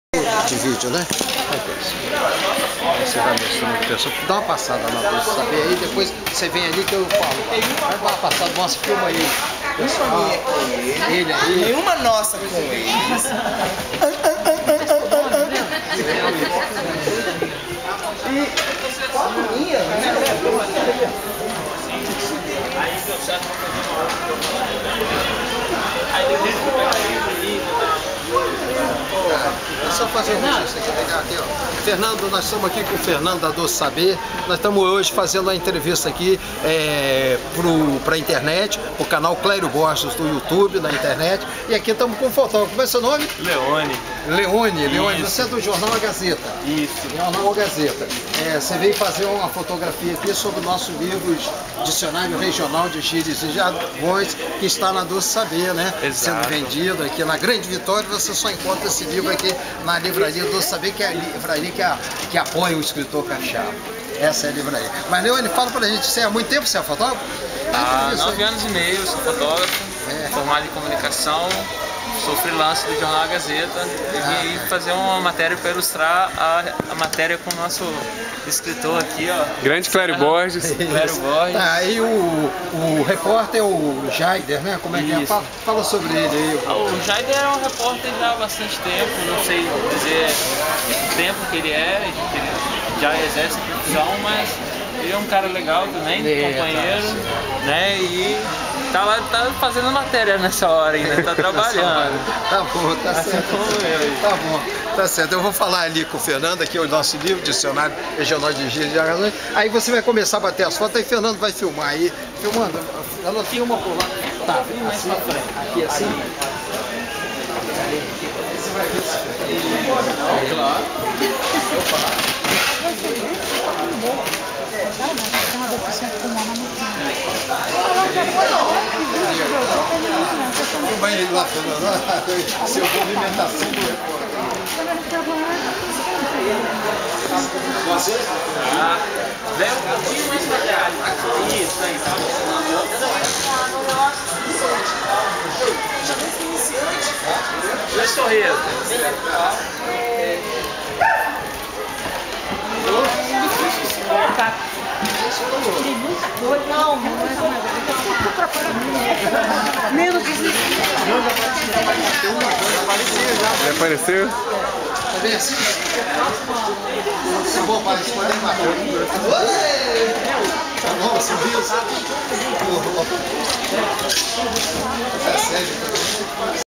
Escritor Clério José Borges sendo fotografado para reportagem no Jornal A Gazeta Vitória ES
Clério José Borges sendo fotografado para reportagem no Jornal A Gazeta Vitória ES Dicionário Regional de Gírias e Jargões de Autoria do Poeta Escritor Historiador Capixaba, Presidente do CTC Clube dos Poetas Trovadores Capixabas, CTC, Clério José Borges. Bienal do Livro Quinta Bienal do Livro no Shopping Norte Sul, Vitória Espírito Santo